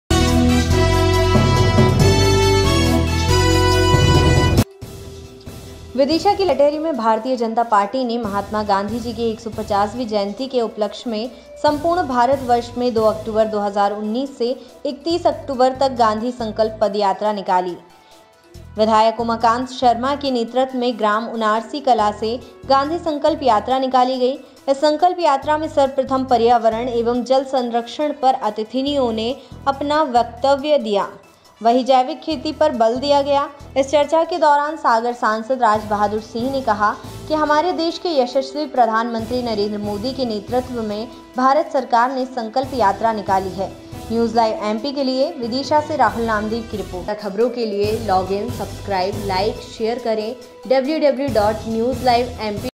भारतीय जनता पार्टी ने गांधी संकल्प पदयात्रा निकाली
विदिशा की लटेरी मे भारतीय जनता पार्टी ने महात्मा गांधी जी की 150वीं जन्म जयंती के उपलक्ष में संपूर्ण भारत वर्ष में 2 अक्टूबर 2019 से 31 अक्टूबर तक गांधी संकल्प पदयात्रा निकाली जा रही है.  विधायक उमाकांत शर्मा के नेतृत्व में ग्राम उनारसी कला से गांधी संकल्प यात्रा निकाली . इस संकल्प यात्रा में सर्वप्रथम पर्यावरण एवं जल संरक्षण पर अतिथियों ने अपना वक्तव्य दिया . वही जैविक खेती पर बल दिया गया इस चर्चा के दौरान सागर  सांसद राज बहादुर सिंह ने कहा कि हमारे देश के यशस्वी प्रधानमंत्री नरेंद्र मोदी के नेतृत्व में भारत सरकार ने संकल्प यात्रा निकाली है.न्यूजलाइवएमपी के लिए विदिशा से राहुल नामदेव की रिपोर्ट